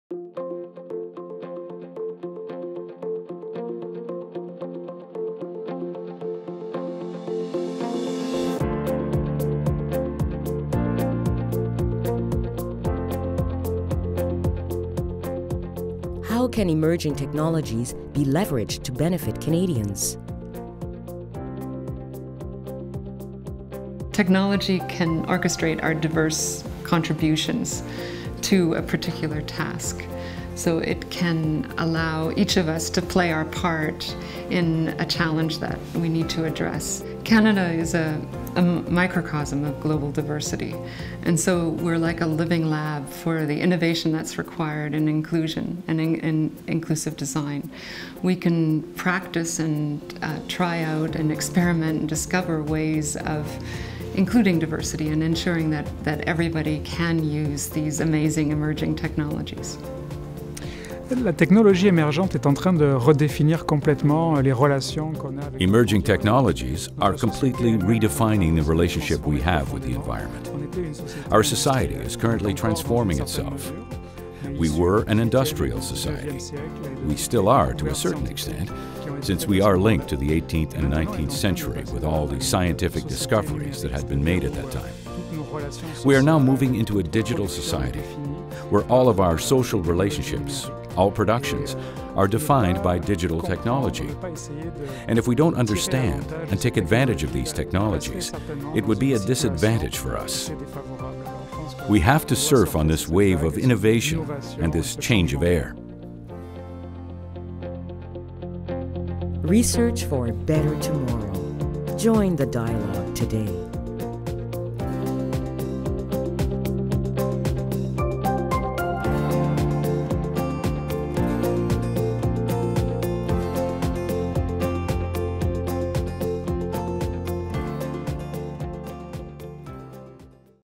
Knowledge Synthesis Grants: Emerging Technologies
How can emerging technologies be leveraged to benefit Canadians?